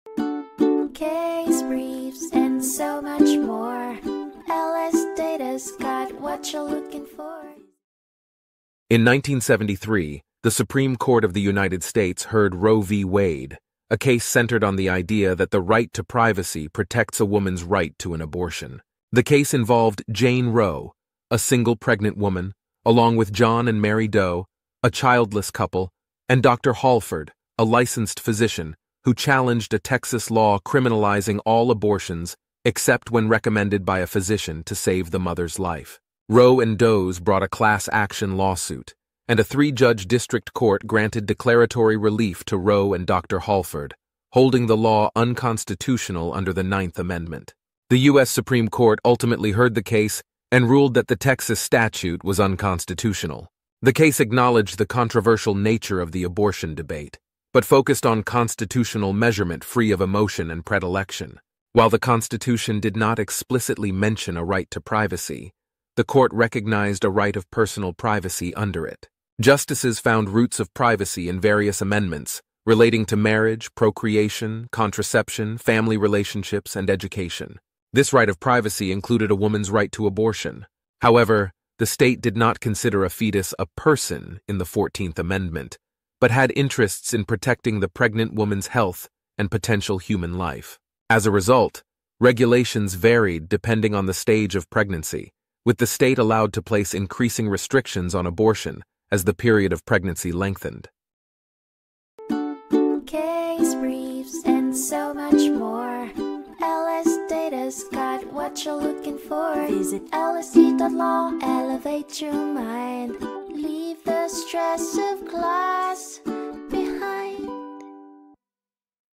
Roe v. Wade (1973) Overview | LSData Case Brief Video Summary
A Texas law made it a crime to “procure an abortion” except “by medical advice for the purpose of saving the life of the mother” Jane Roe, a pregnant single woman, John and Mary Doe, a childless couple, and Dr. Hallford, a licensed physician, all challenged the law.Roe & Does brought a class action lawsuit3-judge district court ruled the Does’ complaint nonjusticiable, but granted declaratory relief to Roe & Dr. Hallford, holding the law unconstitutional under the 9th amendment

Roe v. Wade (1973)
Supreme Court of the United States
410 U.S. 113